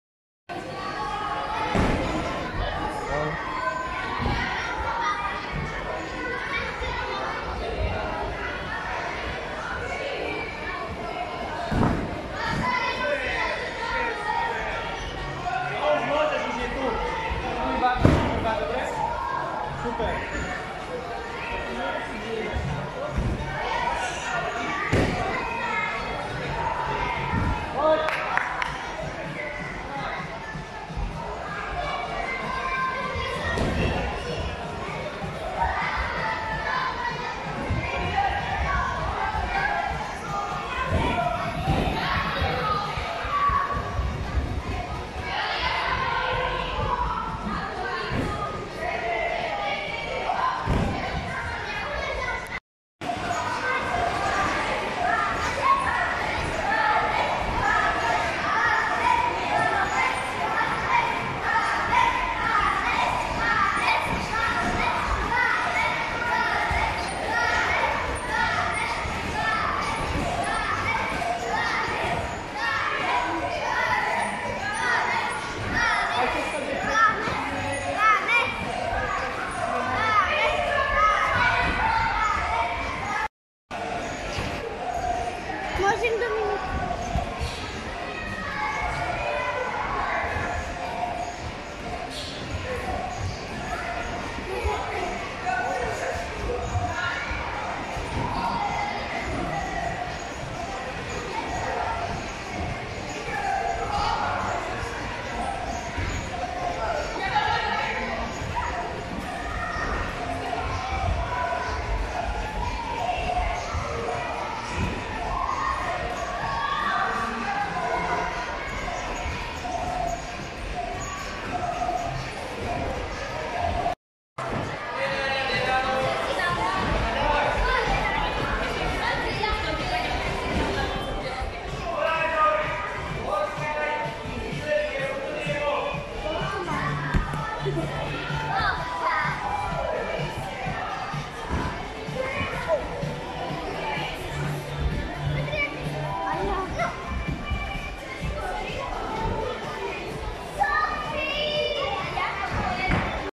JUMP ARENA KOŠICE
Dňa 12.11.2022 si 36 detí v rámci krúžku Spoznávame pohybom užilo deň plný športu a zábavy v Jump Arene v Košiciach. Deti využili 27 trampolín, smečovanie, vybíjanú, penovú jamu, skoky z veže do airbagu, saltá aj chodenie po stene. Žiaci pri rozvíjaní športových zručností a schopností objavovali svoje vlastné limity a odvahu.
                                                                                           E. Rimská, M. Gregová